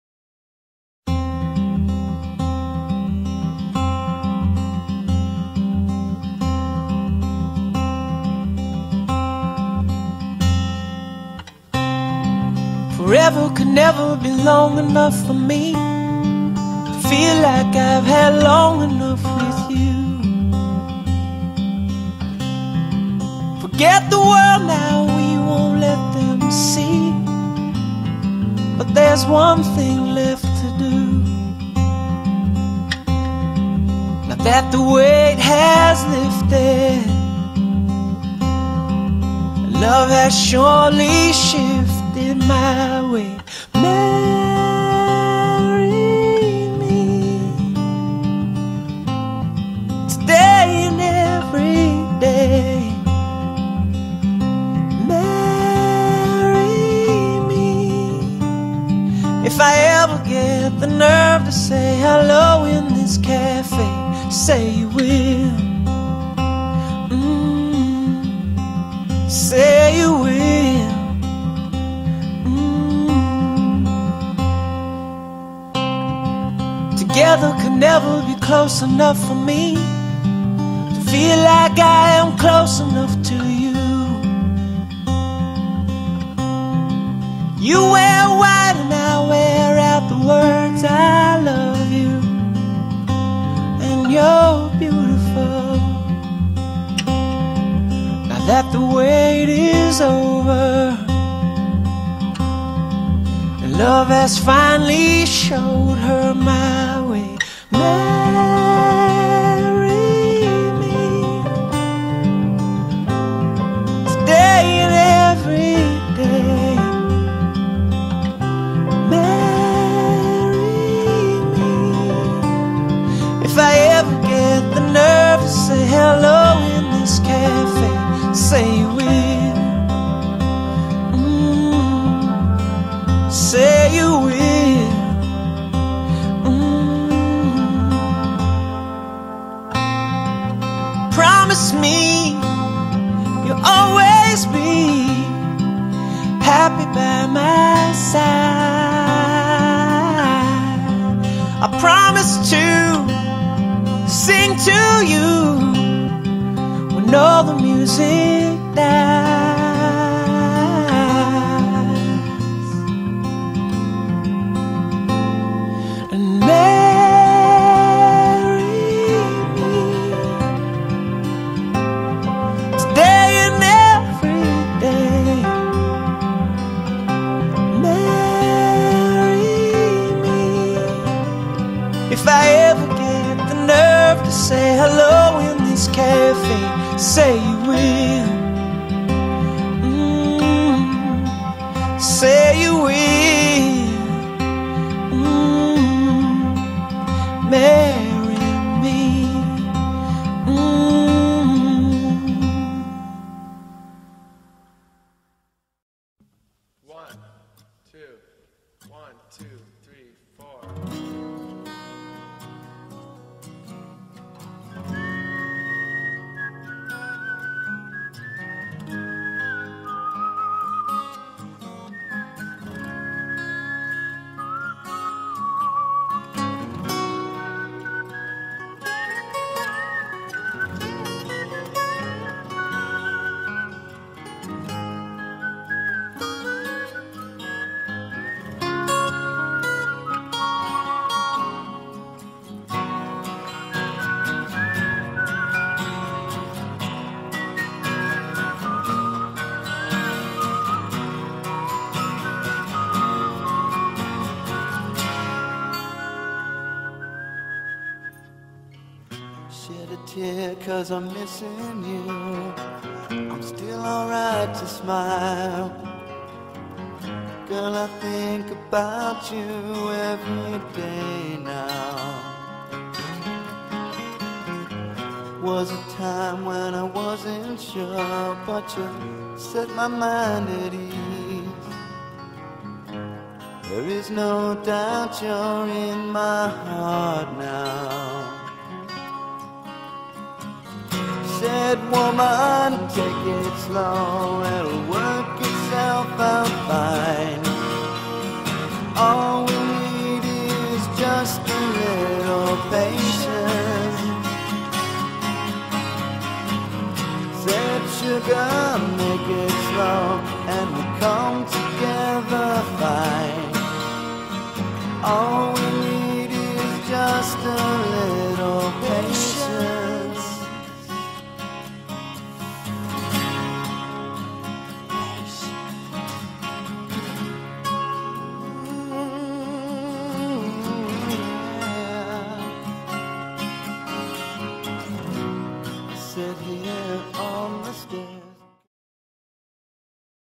Wedding of Tommy & Tiffany at Cavender Castle 8-20-11
North Georgia Wedding of Tommy and Tiffany at Cavender Castle on 8-20-11.